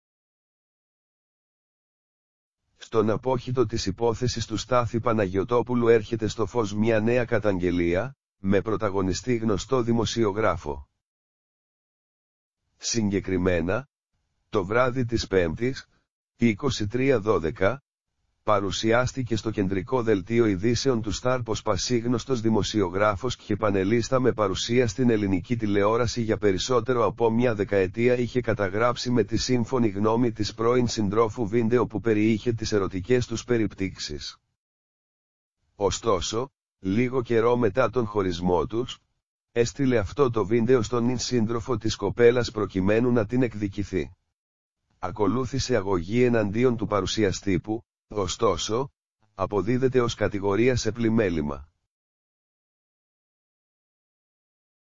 Αγωγή σε γνωστό δημοσιογράφο Διέρρευσε ροζ βίντεο της πρώην συντρόφου του
Στον απόχητο της υπόθεσης του Στάθη Παναγιωτόπουλου έρχεται στο φως μια νέα καταγγελία, με πρωταγωνιστή γνωστό δημοσιογράφο.